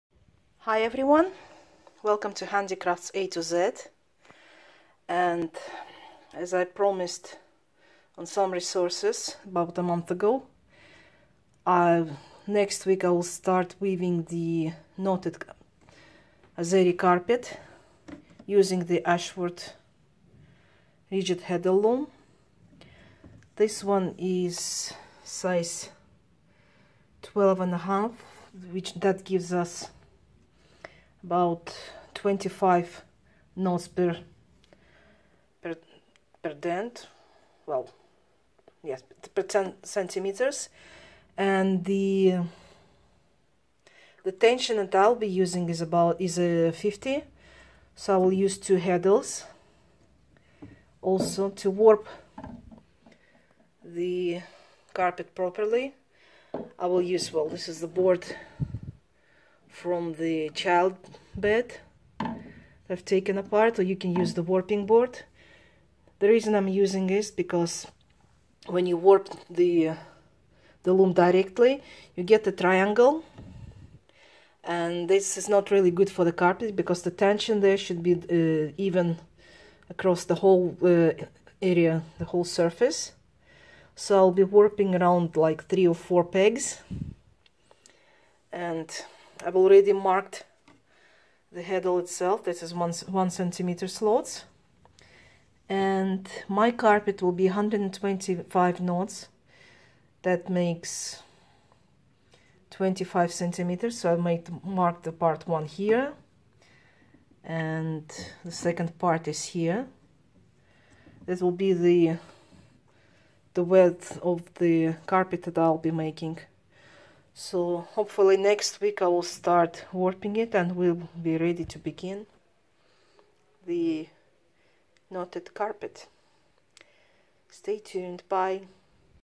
Setting the heddles and warping board for knotted carpet weaving
So I've finally have a setup ready to warp my Ashford loom. To make the right tension of the warp I'll be using a set of pegs, you can use a warping board, to avoid the ends with different length.
Also I've marked the heddles with a ruler and the desired width of a carpet - 125 knots, 50 knots per 10 cm, ~4 inches. Each knot is made over a pair of warp so two heddles give me just the right number of ends - 250.
So next week I'm ready to warp and start weaving a knotted carpet.